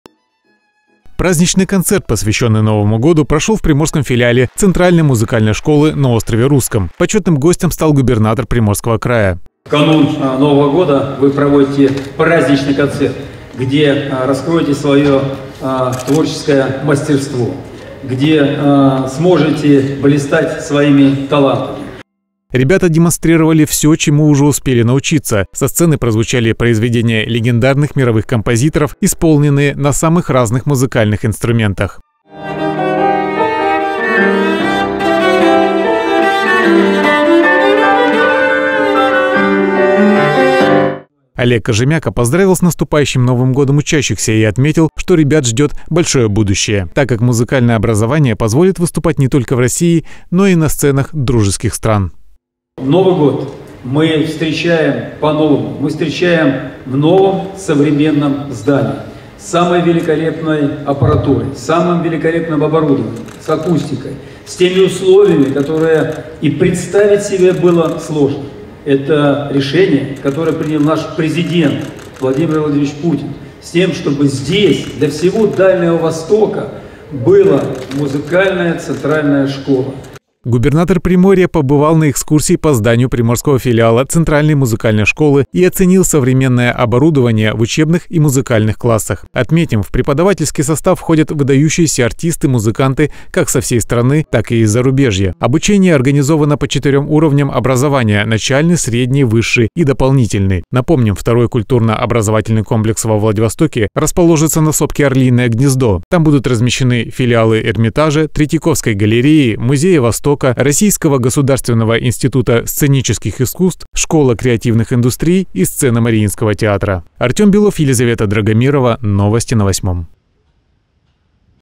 Олег Кожемяко поздравил с наступающим Новым годом учащихся приморского филиала ЦМШ.
Праздничный концерт, посвященный Новому году, прошел в приморском филиале Центральной музыкальной школы на острове Русском. Почетным гостем стал Губернатор край.